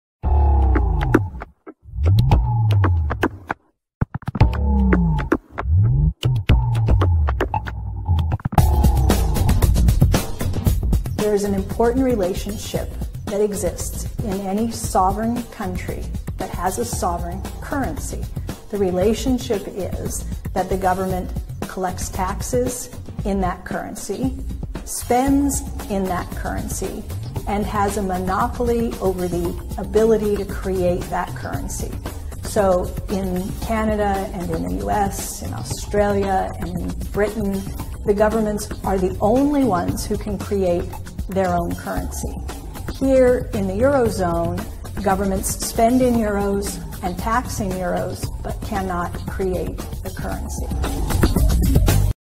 Stephanie Kelton - Stato e Moneta Sovrana
Uno Stato a Moneta Sovrana, Legittimato dai Cittadini, che spenda in Deficit Positivo per il benessere e la Piena Occupazione: è l'Unica Vera Democrazia.
Pillola rossa MMT #2